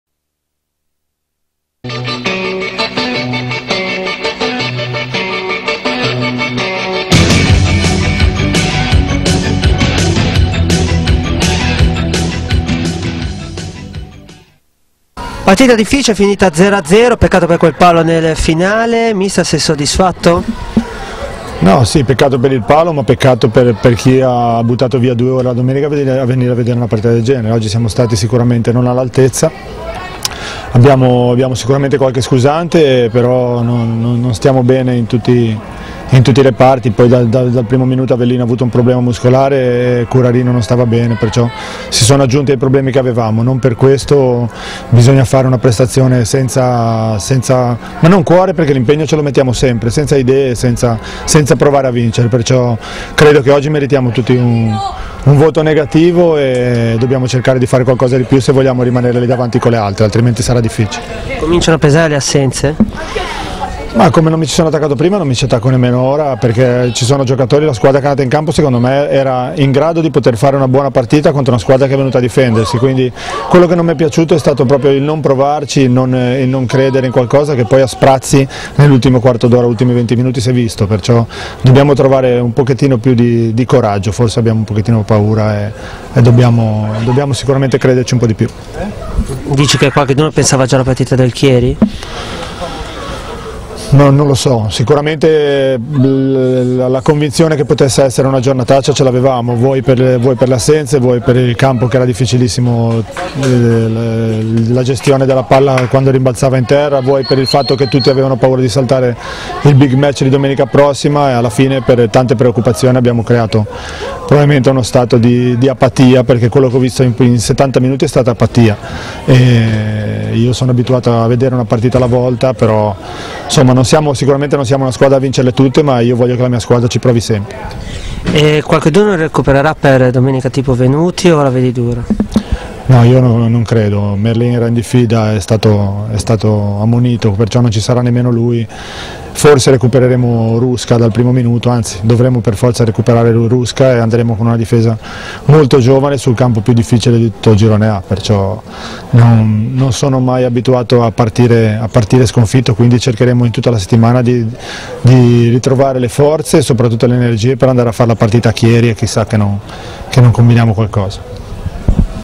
Lavagnese - Folgore Caratese 0-0
Entella TV. Intervista di Joel Roberto Capello, immagini di Carolina Varsi, montaggio di Marco Pinat.
Serie D 2012/2013